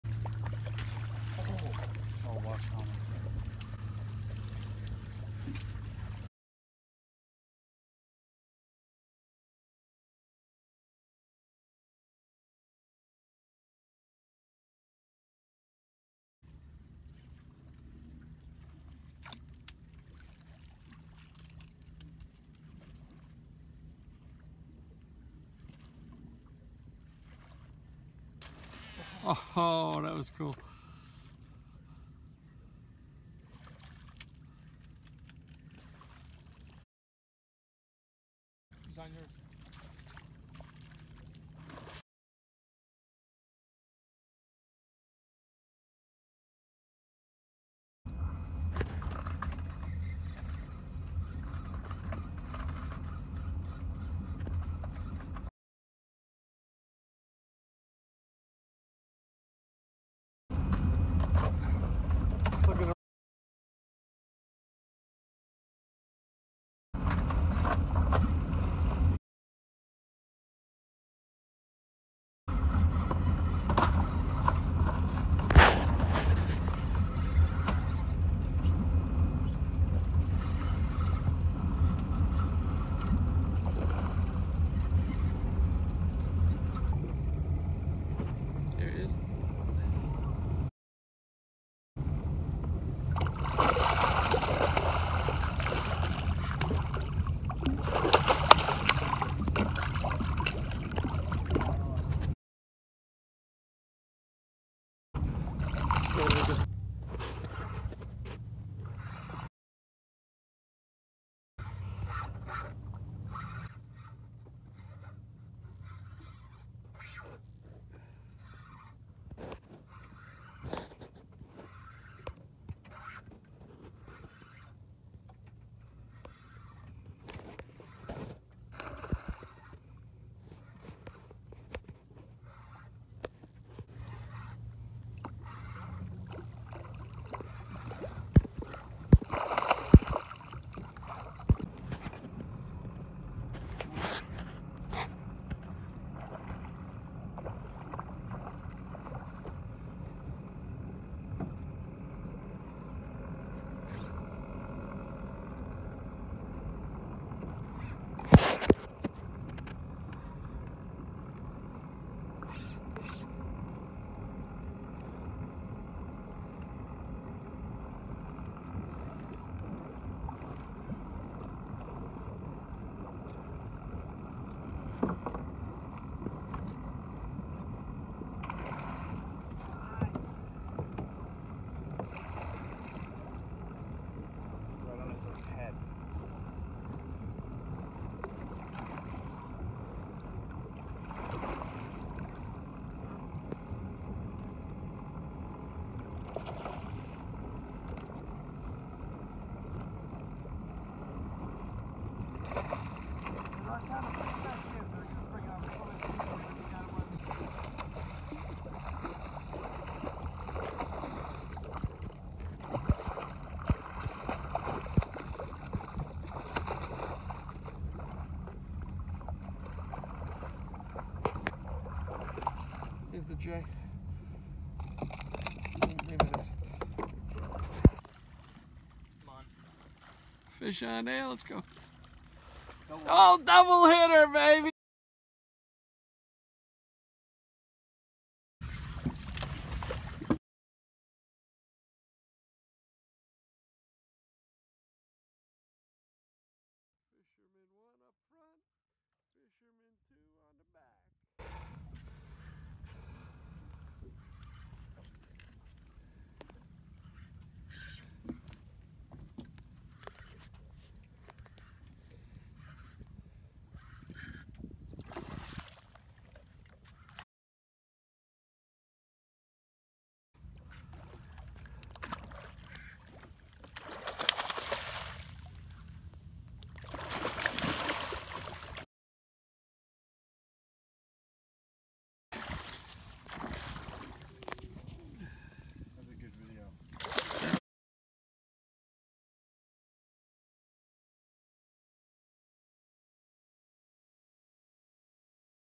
Giant Bluefish hitting topwater during Massive Feeding Frenzy!
Massive 10 - 15 lb Bluefish frenzy attacking bunker pods hitting topwater plugs on Long Island back bays!